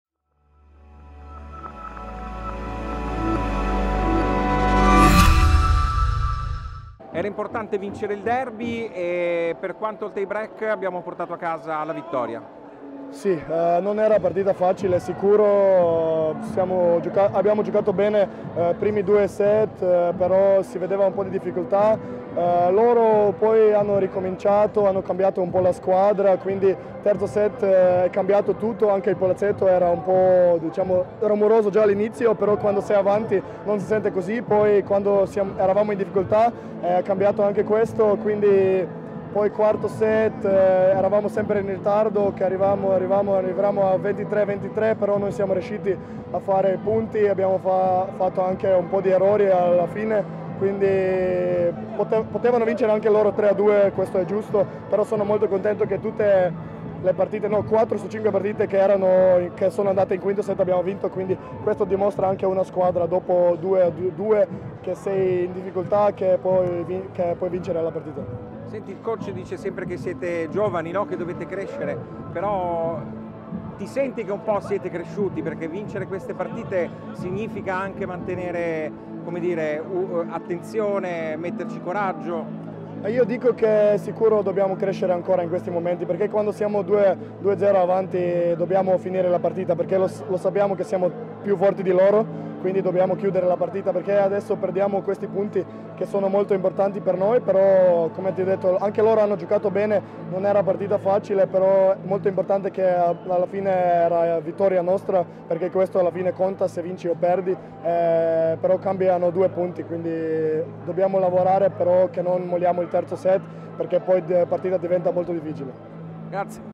WithU Verona: Rok Mozic dopo la vittoria su Padova
Lo schiacciatore sloveno della WithU Verona commenta la vittoria al tie break nel derby veneto con Padova